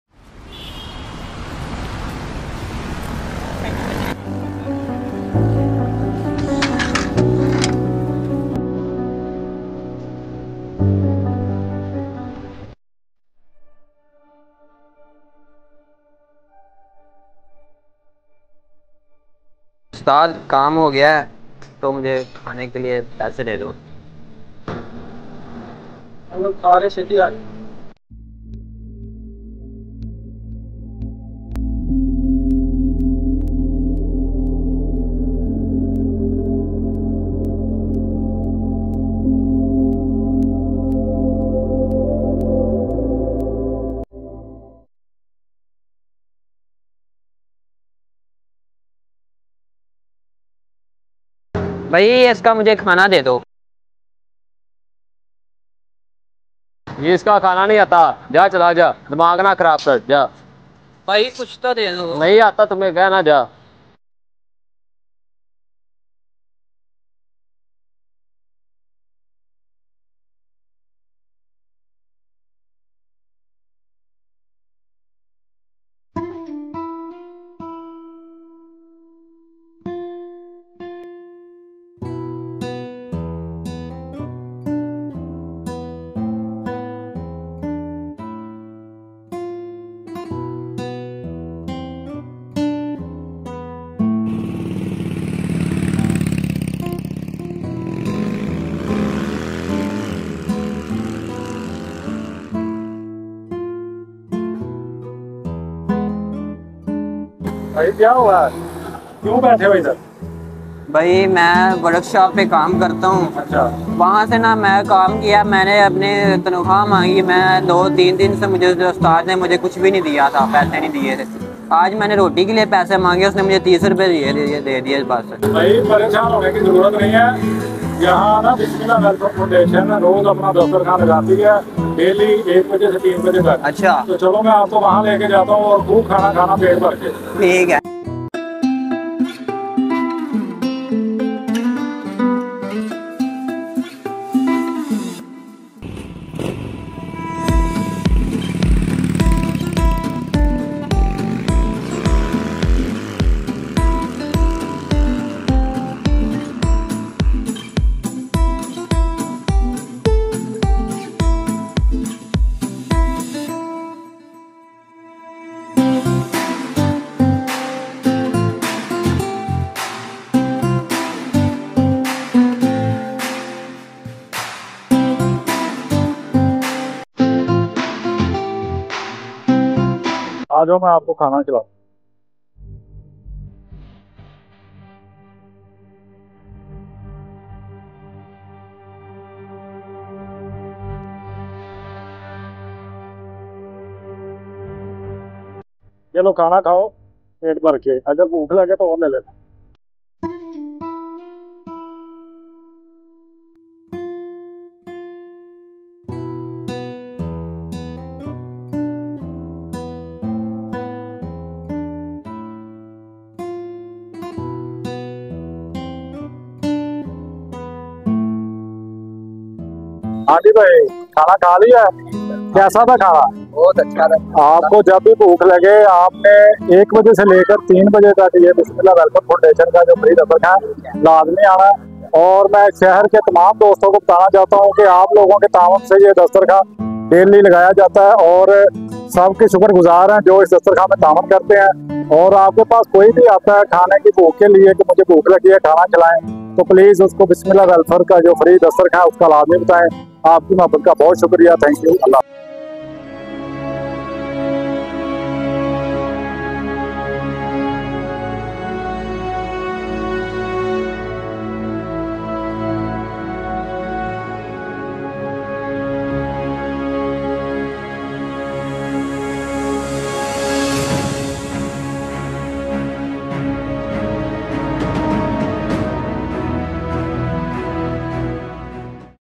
Helping The Hungry: Free Food for Those in Need
In this video, we give free meals to people who need them most. Watch how a simple act of kindness can make a big difference in someone’s life. Join us in spreading love and care to those who are hungry. Use FeedTheNeedyVlogs to support our cause!